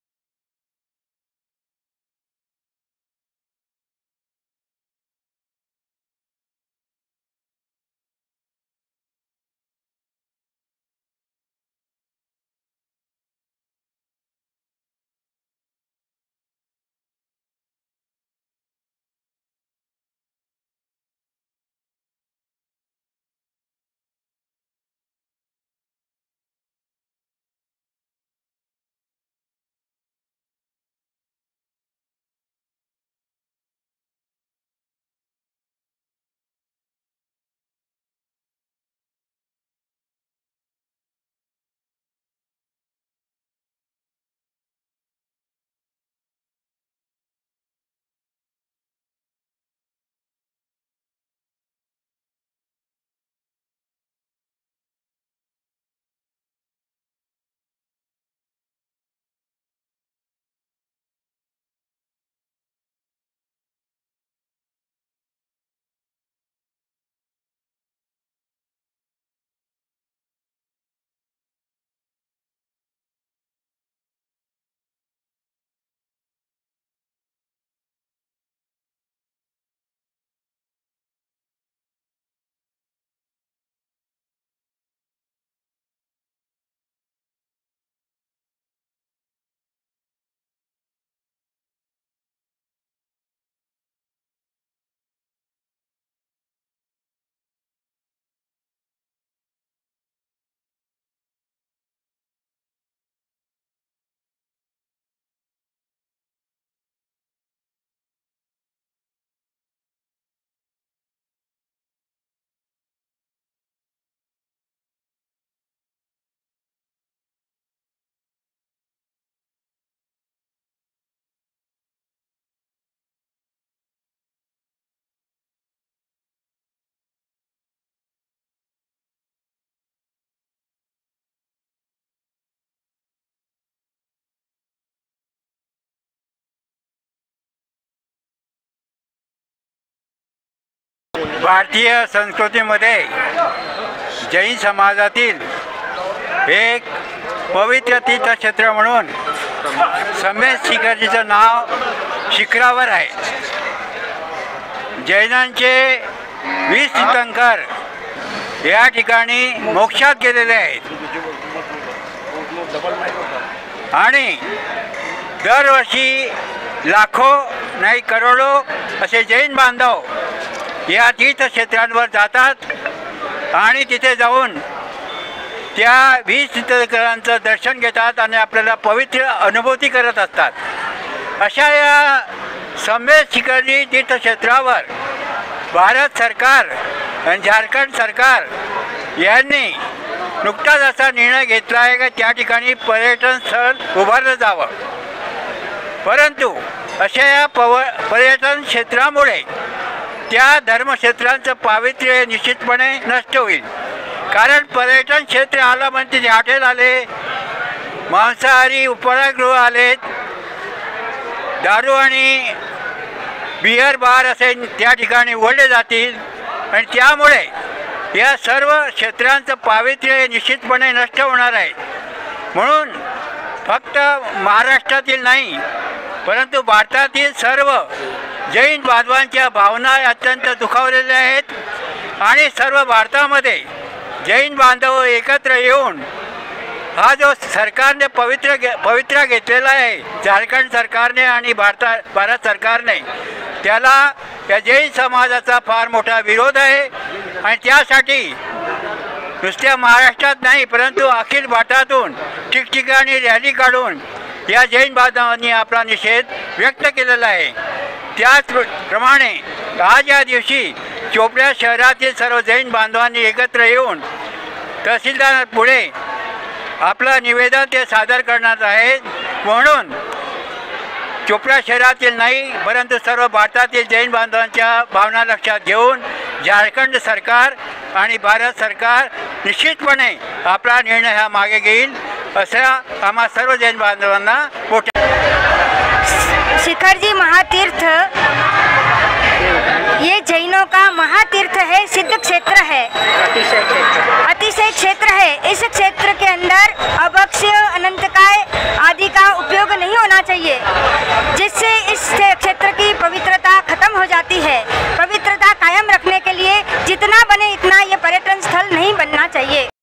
JALGAON | पर्यटनस्थळचा दर्जा देण्याच्या प्रस्तावा विरोधात सकल जैन समाजाच्या वतीने मूक मोर्चा
झारखंड सरकार व भारत सरकारने श्री सम्मेद शिखर जी तीर्थक्षेत्राला पर्यटन स्थळाच्या दर्जा देऊन विकसित करण्याचा प्रस्तावाला सकल जैन समाजाच्या वतीने विरोध करण्यात आलेला आहे. जैन समाजातील पवित्र तीर्थक्षेत्र म्हणून सम्मेद शिखरजी चे नाव शिखरावर आहे दरवर्षी करोडो जैन  बांधव यां तीर्थक्षेत्रावर जातात आणि दर्शन घेतात अशा या सम्मेद शिखरजी तीर्थक्षेत्रावर भारत सरकार आणि झारखंड सरकार यांनी नुकताच असा निर्णय घेतला या ठिकाणी पर्यटन स्थळ उभारले जावं परंतु अशा या पर्यटन क्षेत्रामुळे त्या धर्मक्षेत्राचे  पावित्र्य निश्चितपणे नष्ट होईल या निर्णयामुळे भारतातील सर्व जैन  बांधवांचे भावना अत्यंत दुखावलेले आहे. झारखंड सरकार आणि भारत सरकार यांच्या निर्णयाला जैन समाजाचा  फार मोठा विरोध आहे संपूर्ण भारतभर जैन समाजाने ठिकठिकाणी मोर्चा काढून निषेध नोंदविला आहे. संपूर्ण भारतातील जैन बांधवांचे भावना लक्षात घेऊन झारखंड सरकार आणि भारत सरकार आपला हा निर्णय मागे घेईल अशी अपेक्षा व्यक्त केली सदर चोपडा शहरातील गोल मंदिर परिसरापासून मेन रोडाने पाठक यांनी गांधी चौक मार्ग नियुक्त कार्यालयावर मोर्चा आला या मोर्चामध्ये सकल जैन समाजाचे नागरिक महिला मोठ्या संख्येने सहभागी झाले होते .

बाईट -   प्रा  शांतीलाल भोथरा
बाईट-   साध्वी.